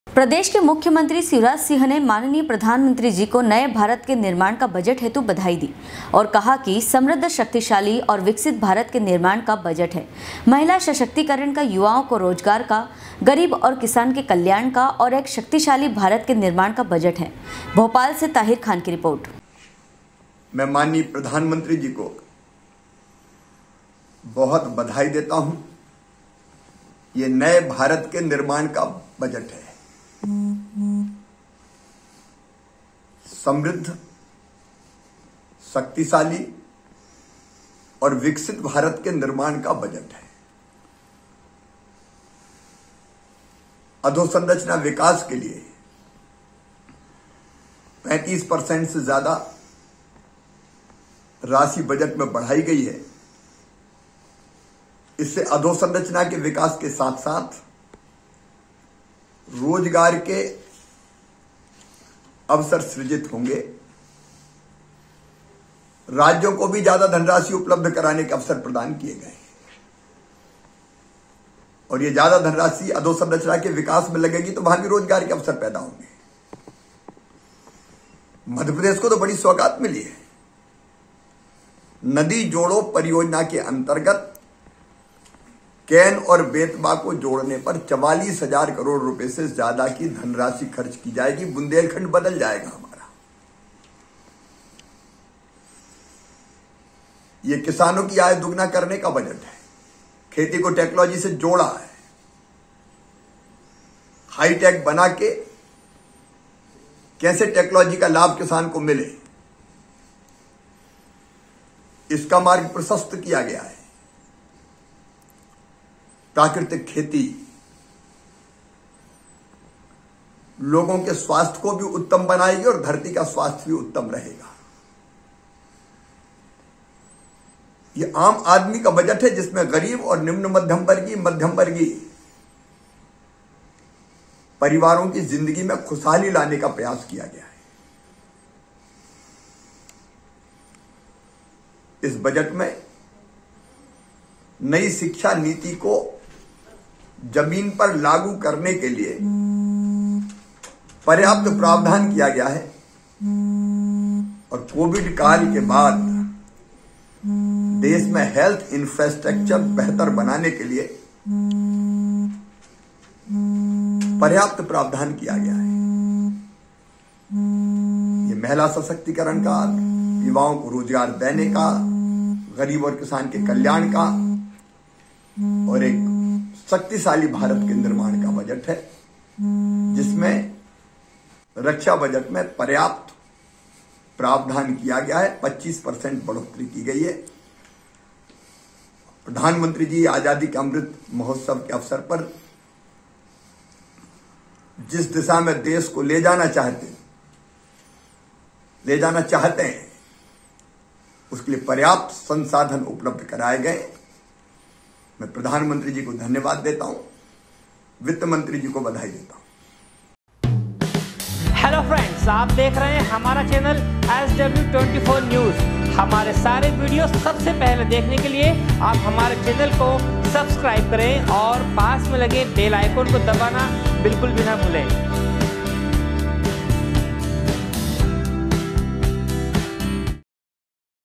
SW 24 News Madhyapradesh
प्रदेष के मुख्यमंत्री षिवराज सिंह ने माननीय प्रधानमंत्री जी को नए भारत के निर्माण का बजट हेतु बधाई दी और कहा की समृद्ध शक्तिशाली और विकसित भारत के निर्माण का बजट है महिला सशक्तिकरण का युवाओं को रोजगार का गरीब और किसान के कल्याण का और एक शक्तिशाली भारत के निर्माण का बजट है। भोपाल से ताहिर खान की रिपोर्ट